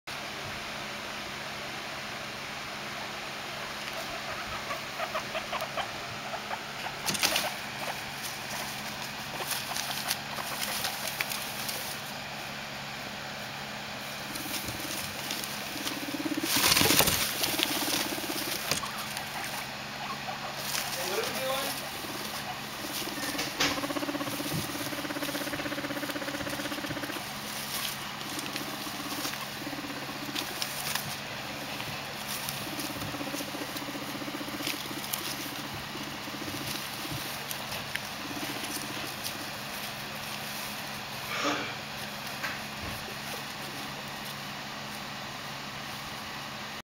Hamstorr puts Bear in his place
Bear goes up the ramp to go home, finds Hamstorr there, is overjoyed as always, but Hamstorr can't let Bear get too carried away...